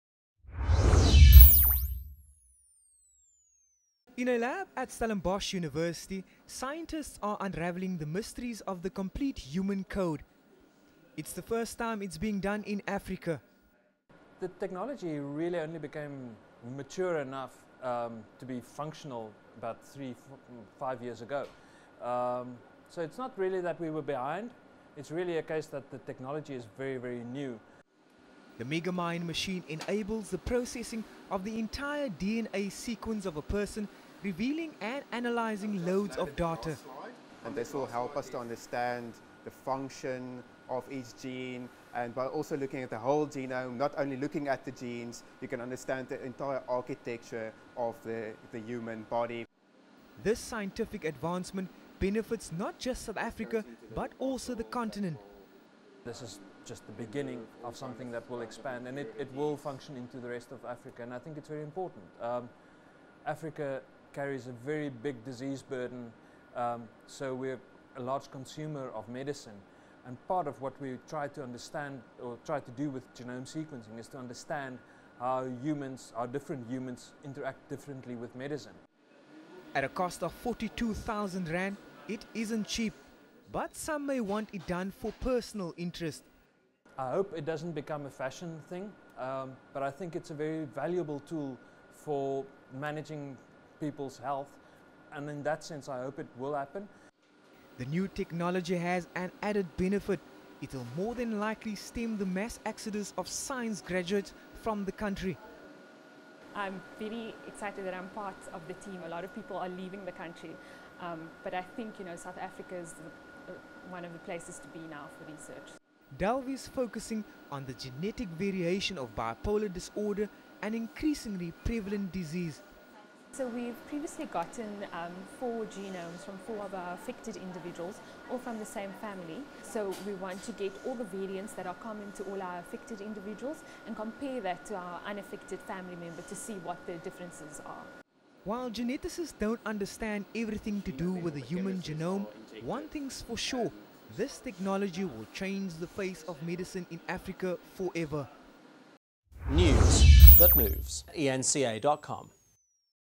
Entire human genome gets broken down in SA
Johannesburg, September 18 -- In a first for Africa, the entire human genome has been broken down and analysed on South African soil.  It comes 10 years after the International Genome Project was completed.  The genome contains all the body's hereditary information, including genes and DNA.